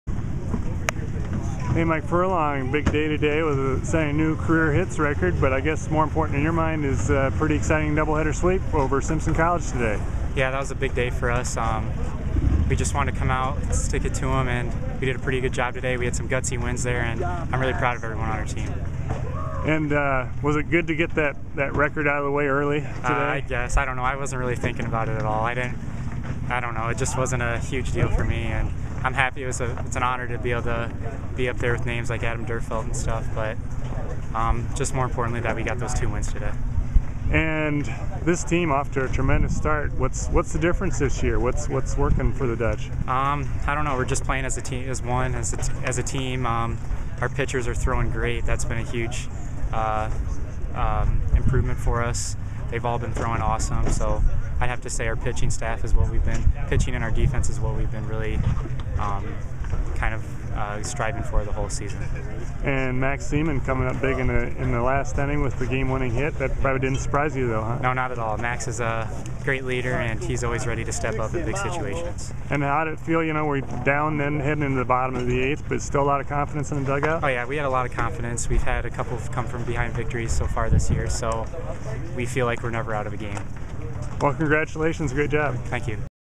Central College shortstop Mike Furlong
Central College shortstop Mike Furlong talks about his team's baseball sweep of Simpson College, winning 7-2 and 4-3 in 8 innings. Furlong had two hits to give him 209 for his career, breaking the school record set by Adam Duerfeldt (2003-06).